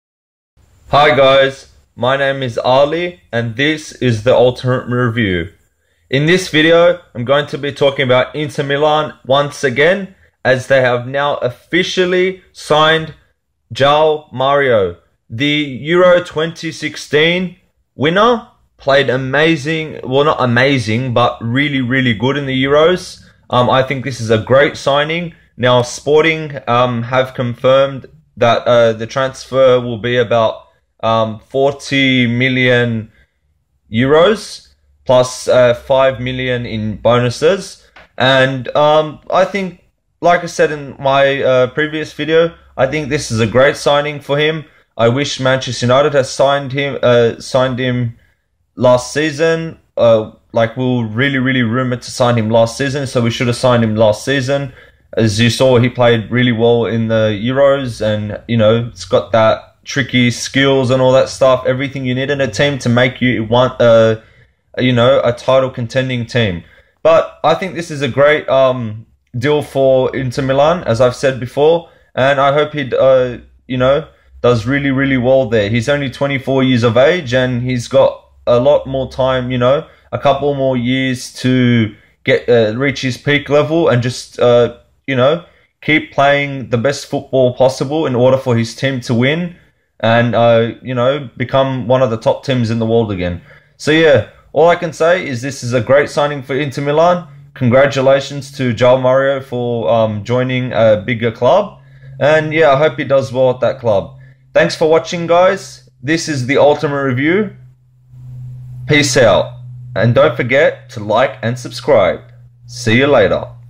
INTER MILAN HAVE SIGNED JOAO MARIO FOR €45 MILLION
Sporting CP have announced the transfer of Joao Mario to Inter for €40m plus €5m bonuses.

The midfielder flew in for his medical yesterday and worked out personal terms.

The only issue was to seal the system of payment, as initially it was reported Inter wanted a €10m loan with €35m obligation to buy in 2017.

Instead, the statement from Sporting reads that “we have reached an agreement with Inter Milan for the transfer of the player Joao Mario for €45m, of which €5m is dictated by objectives.

“The club wishes Joao Mario happiness on a personal and professional level.”